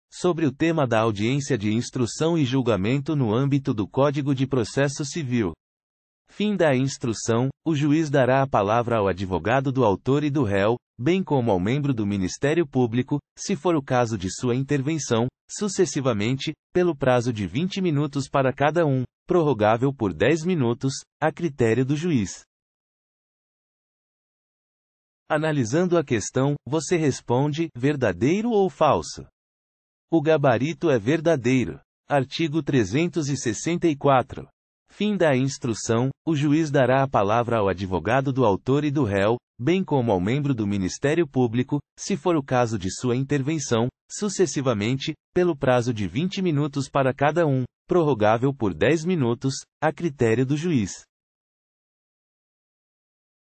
QScon Direito: [Art. 364, CPC] FMP Concursos 2017 - Procurador do Estado (PGE-AC] - Adaptada
Art. 364, CPC | FMP Concursos 2017 | Procurador do Estado | (PGEAC | Adaptada

Sobre o tema da audiência de instrução e julgamento no âmbito do Código de Processo Civil. Finda a instrução, o juiz dará a palavra ao advogado do autor e do réu, bem como ao membro do Ministério Público, se for o caso de sua intervenção, sucessivamente, pelo prazo de 20 minutos para cada um, prorrogável por 10 minutos, a critério do juiz.

Gabarito: Verdadeiro